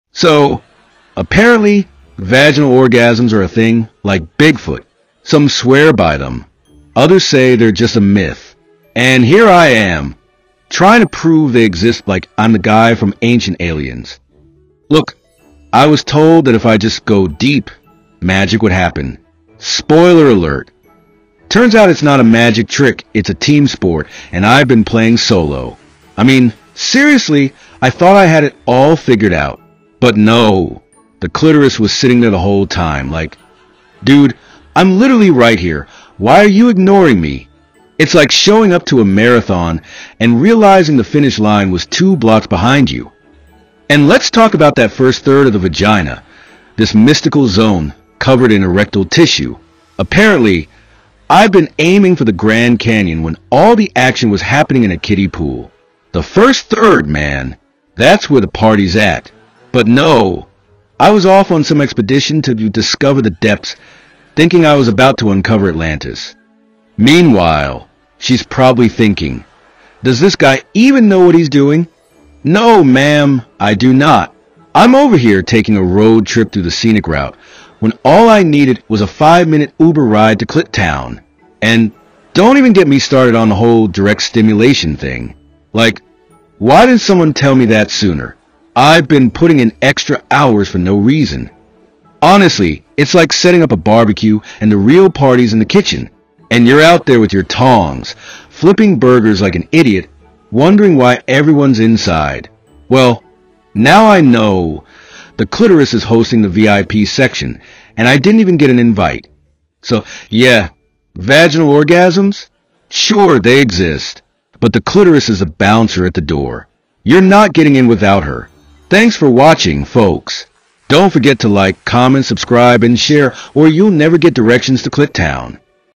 The Great Vaginal Orgasm Debate: A Guy’s Guide to Missing the Mark
Description: "Join us in this comedic yet educational journey about the ongoing debate of vaginal orgasms versus clitoral stimulation. Learn why the clitoris is the real MVP in the bedroom, and how to navigate sexual pleasure with humor and confidence."